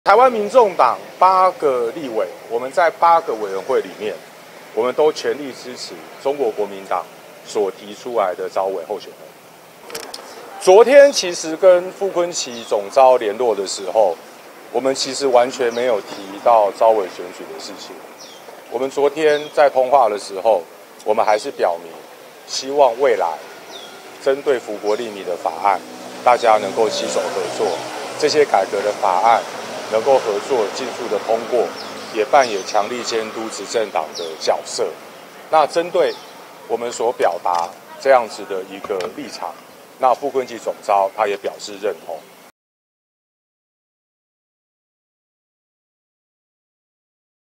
立院召委今選舉 黃國昌：民眾黨全力支持國民黨團人選
立法院8個常設委員會今（29）日舉行召委選舉，民眾黨團上午舉行選前會議，總召黃國昌表示，會議決議召委選舉中，「台灣民眾黨8個立委，在8個委員會裡面，都全力支持國民黨所提出來的召委候選人」。黃國昌說，昨與國民黨團總召傅崐萁通話，完全沒有提及召委選舉的事情，雙方僅表明希望未來可以針對福國利民的法案攜手合